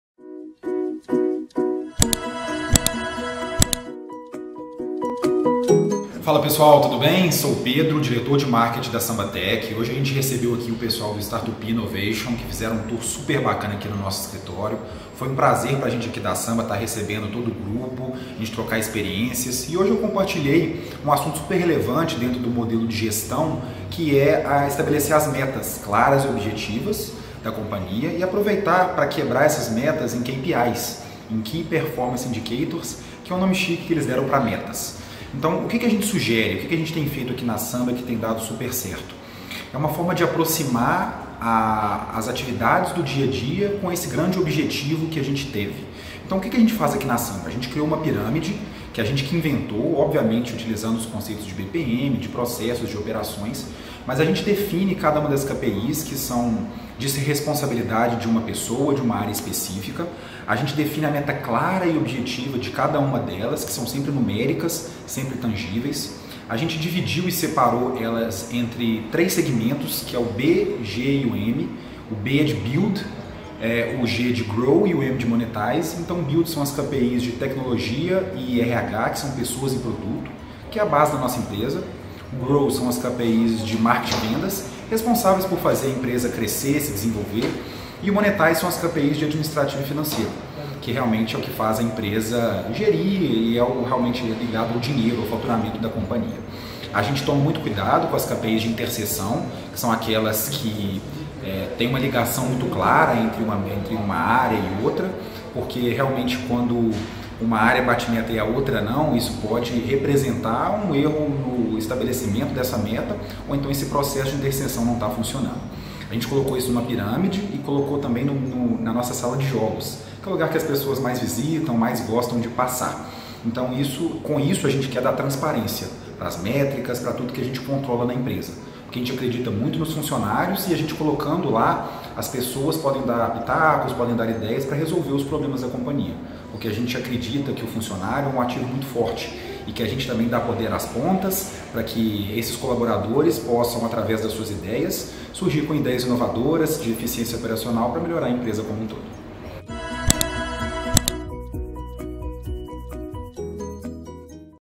Diretor de Marketing da Sambatech dá dicas sobre KPI
O Startupi Innovation Tour desembarcou em Belo Horizonte e uma das empresas que recebeu nosso grupo de braços abertos foi a Sambatech.
Pedro Filizzola, diretor de marketing da startup, bateu um papo super bacana com os participantes e deu dicas valiosas sobre gestão e estabelecimento de metas. Quer saber mais? Assista!